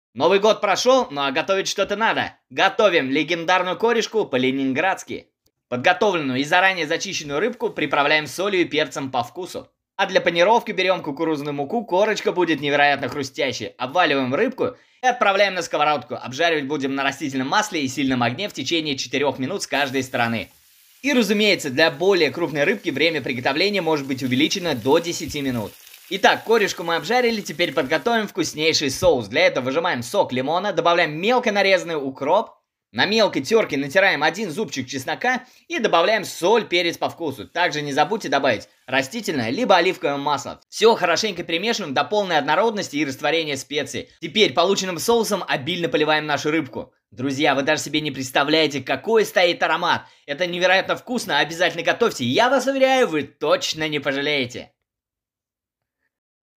Корюшка по-ленинградски
ИНГРЕДИЕНТЫ:
🌶Корюшка 700г
🌶Мука кукурузная 2ст л

🌶Сок лимона 1/3шт
🌶Масло раст 2ст
🌶Укроп 10г